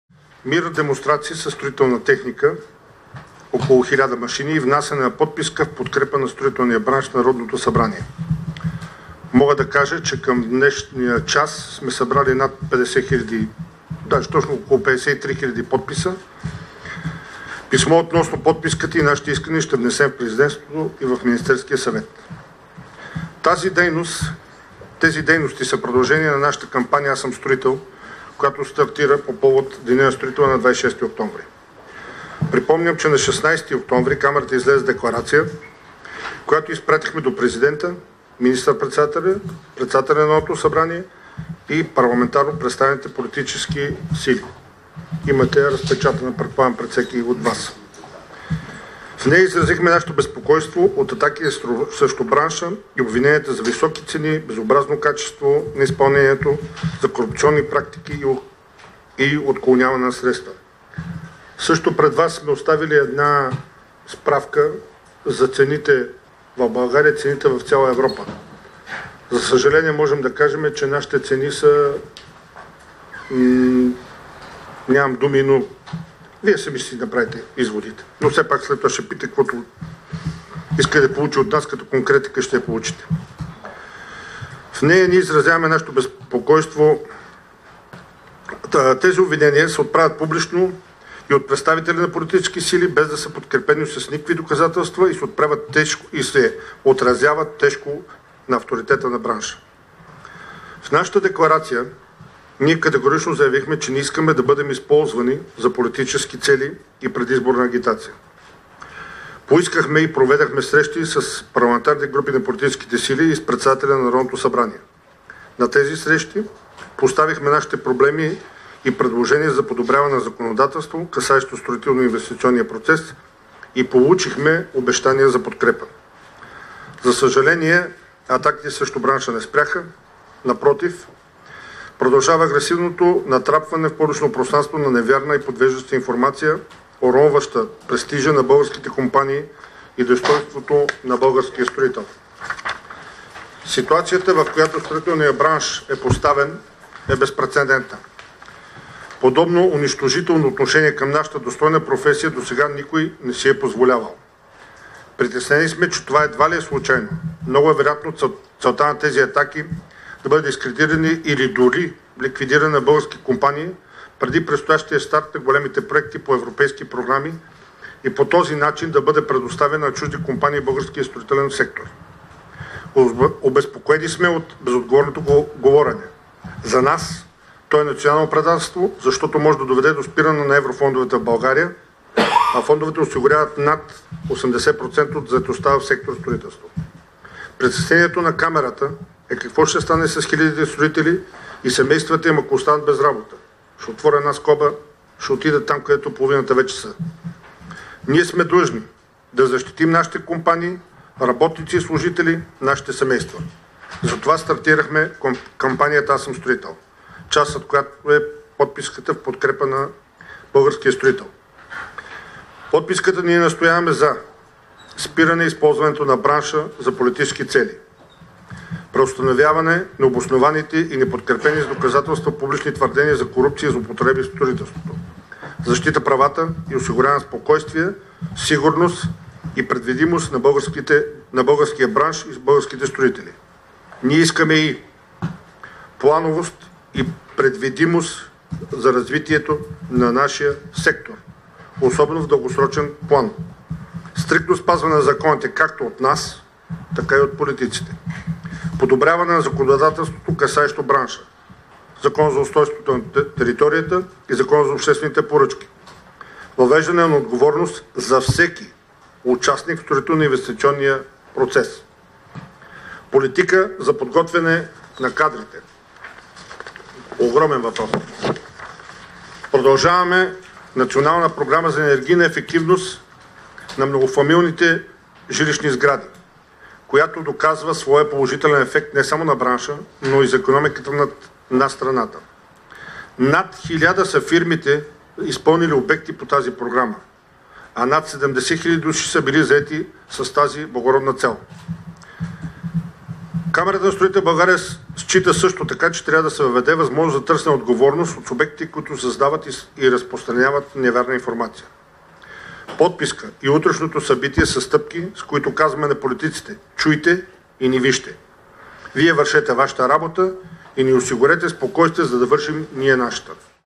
Строителният бранш излиза на мирен протест, поне засега
На 12 декември строителите ще изкарат 1000 машини строителна техника по време на мирна демонстрация. Това заяви председателят на УС на Камарата на строителите в България Илиян Терзиев. /БГНЕС